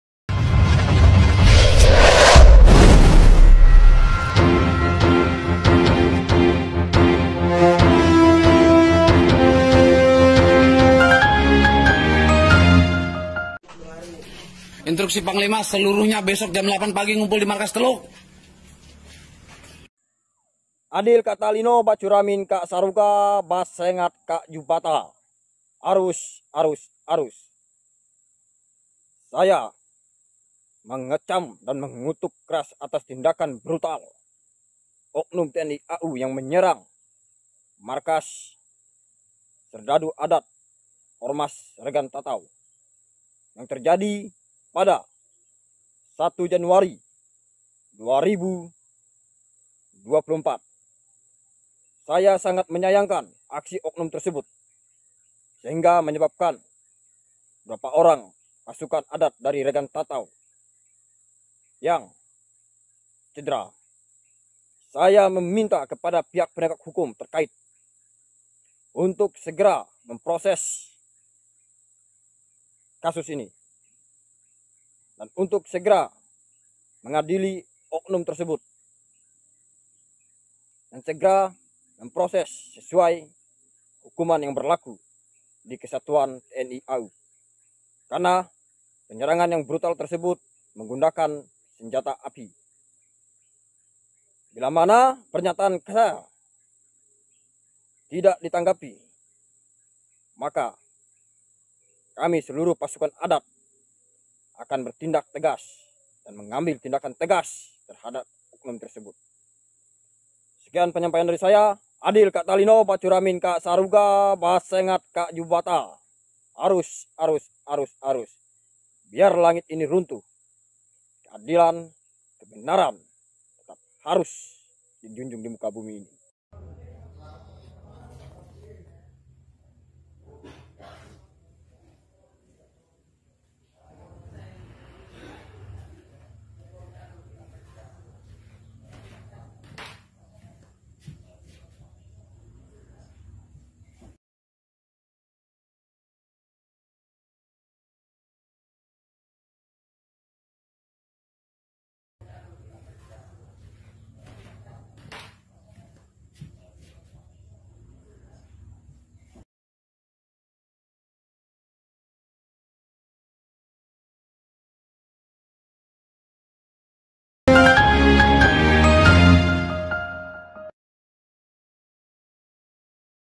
VIRAL PERNYATAAN TEGAS PANGLIMA PAJAJI
VIRAL PERNYATAAN TEGAS PANGLIMA PAJAJI panglimapajaji pernyataan @eyangboyart6414

kembali viral, pernyataan tegas terbuka panglima pajaji,terdahap pihak aparat yang telah melakukan penyerangan dan pengrusakan markas serdaduoq adat regant tatau Kalimantan.